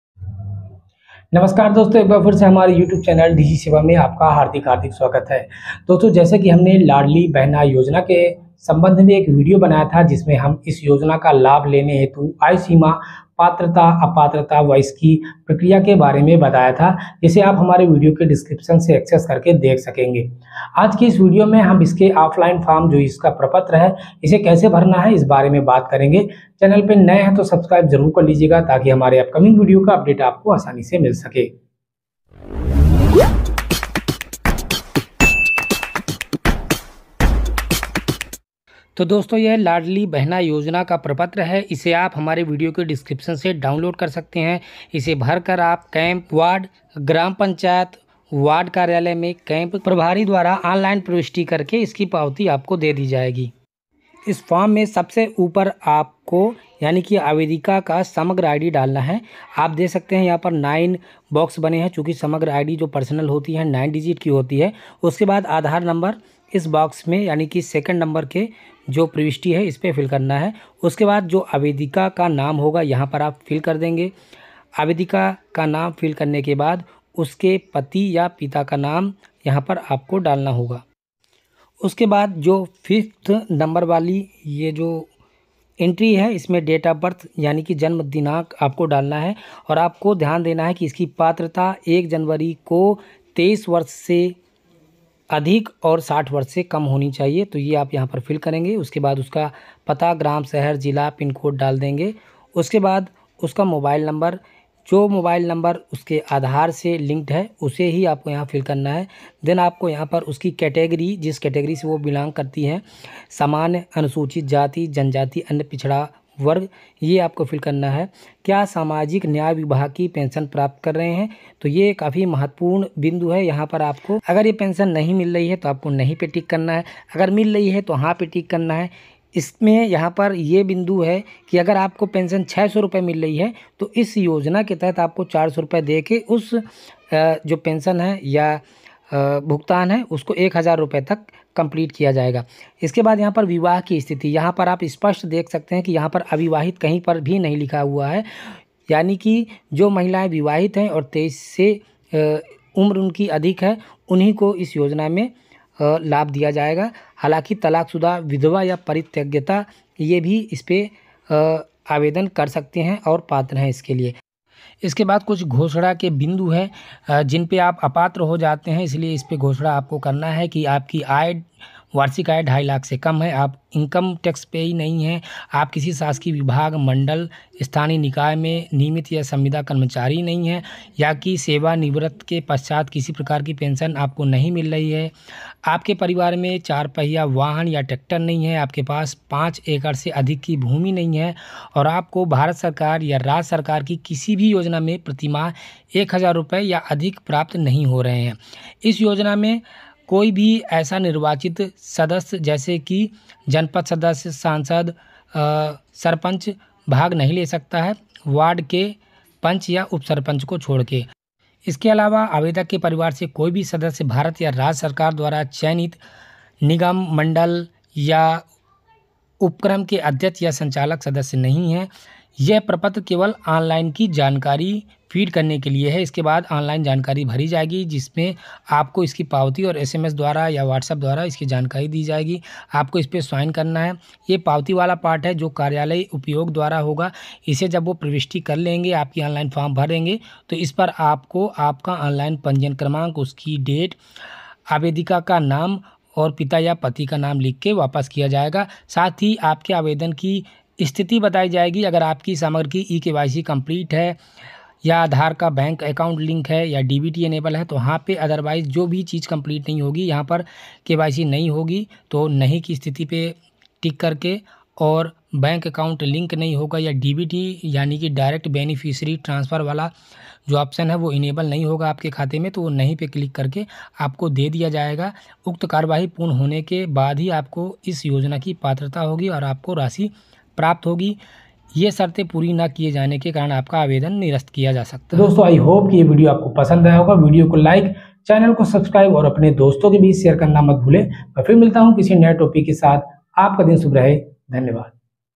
लाडली बहना आवेदन फॉर्म कैसे भरे MP Ladli Bahna Yojna 2023 l Download Ladli Yojna Form
इस वीडियो में मध्य प्रदेश माननीय मुख्यमंत्री शिवराज सिंह चौहान द्वारा बहु प्रतीक्षित योजना लाडली बहना योजना 2023 के बारे में उसकी पात्रता आयु एवं संपूर्ण प्रक्रिया एवं योजना के क्रियान्वयन से संबंधित जानकारी प्रदान की गई है.